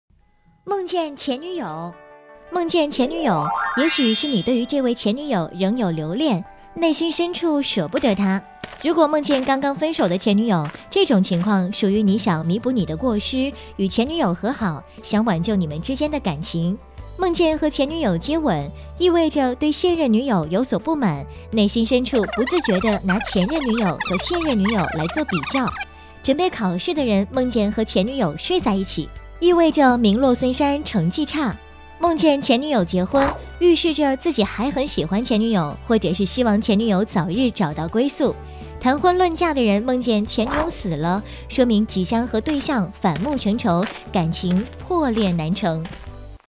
夢見前女友是什么意思
夢見前女友，也許是你對于這位前女友仍有留戀，內心深處舍不得她。如果夢見剛剛分手的前女友，這種情況屬于你想彌補你的過失，與前女友和好，想挽救你們之間的感情。　　夢見和前女友接吻，意味著對現任女友有所不滿，內心深處不自覺的拿前任女友和現任女友來做比較。準備考試的人夢見和前女友睡在一起，意味著名落孫山。成績差。　　夢見前女友結婚，預示著自己還很喜歡前女友或者是希望前女友早日找到歸宿。 談婚論嫁的人夢見前女友死了，說明即將和對象反目成仇，感情破裂難成。